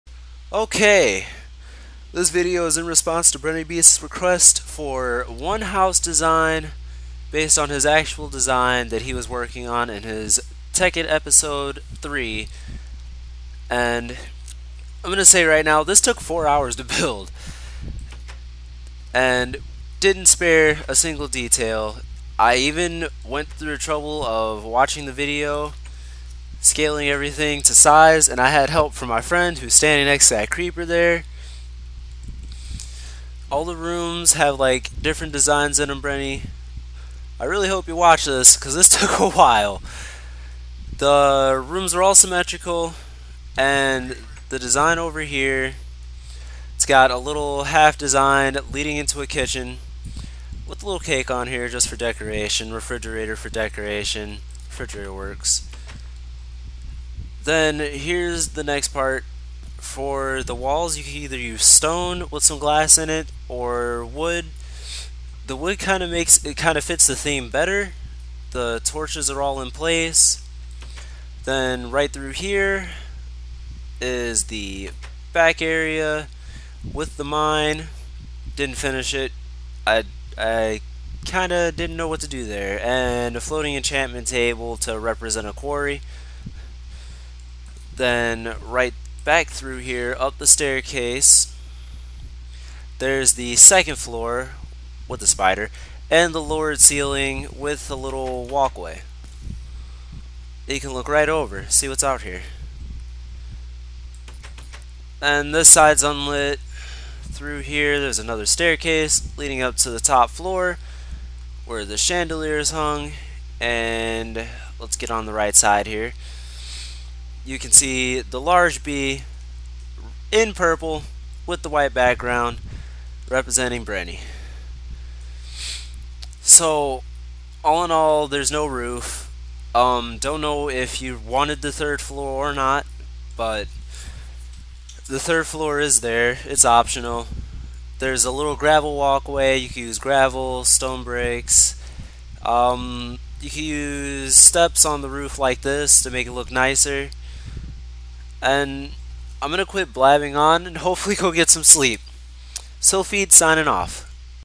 [Breny] Home Design
Took four hours of building, but made a house like that which was requested to have specific features by BrenyBeast.